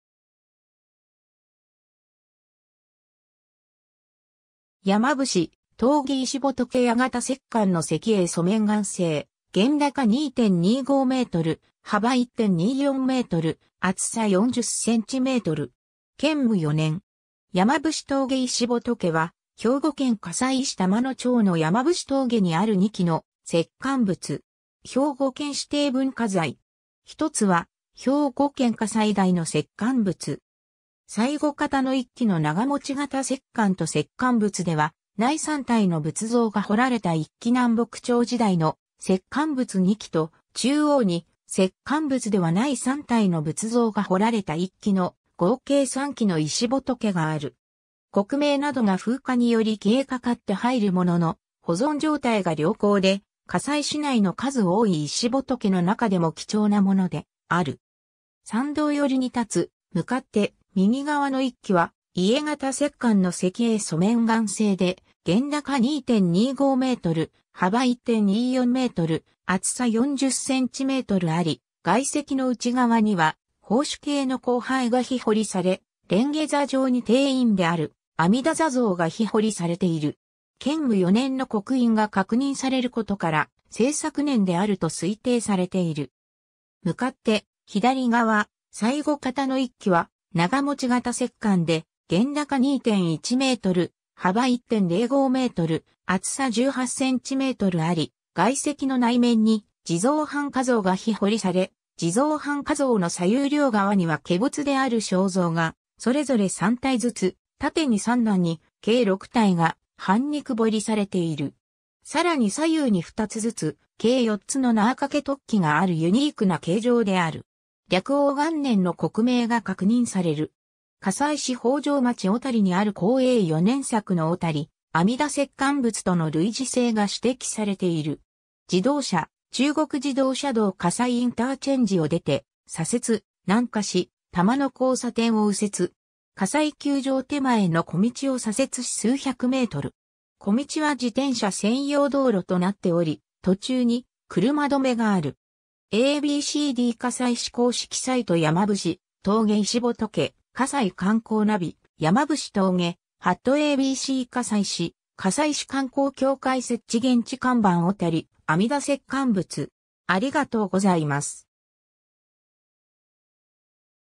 山伏峠石仏
山伏峠石仏 家型石棺の石英粗面岩製、現高2.25m、幅1.24m、厚さ40cm。
建武4年（1337年）。
 山伏峠石仏（やまぶしとうげせきぶつ）は、兵庫県加西市玉野町の山伏峠にある2基の石棺仏。
兵庫県指定文化財。
ひとつは兵庫県下最大の石棺仏。
 最後方の1基の長持型石棺（現高2.1m、幅1.05m、厚さ18cm）と石棺仏ではない3体の仏像が彫られた1基 南北朝時代の石棺仏2基と、中央に石棺仏ではない3体の仏像が彫られた1基の合計3基の石仏がある。
刻銘などが風化により消えかかって入るものの、保存状態が良好で加西市内の数多い石仏の中でも貴重なものである。
 山道寄りに立つ向かって右側の1基は、家型石棺の石英粗面岩製で、現高2.25m、幅1.24m、厚さ40cmあり、蓋石の内側には宝珠形の光背が陽彫され、蓮華座上に定印である阿弥陀座像が陽彫されている。
建武4年（1337年）の刻印が確認されることから、制作年であると推定されている。
 向かって左側、最後方の1基は長持型石棺で、現高2.1m、幅1.05m、厚さ18cmあり、蓋石の内面に地蔵半跏像が陽彫され、地蔵半跏像の左右両側には化仏である小像がそれぞれ3体ずつ、縦に3段に計6体が半肉彫りされている。
さらに左右に2つずつ、計4つの縄かけ突起があるユニークな形状である。
暦応元年(1338年)の刻銘が確認される。
 加西市北条町小谷にある康永4年（1345年）作の小谷阿弥陀石棺仏（よばりこき地蔵）との類似性が指摘されている。
 自動車:中国自動車道加西インターチェンジを出て左折、南下し、玉野交差点を右折。
加西球場（アラジンスタジアム）手前の小道を左折し数100m。
小道は自転車専用道路となっており、途中に車止めがある。
 ^ a b c d 加西市公式サイト"山伏峠石仏" ^ かさい観光Navi「山伏峠」 ^ a b c 加西市・加西市観光協会設置 現地看板 ^ 小谷阿弥陀石棺仏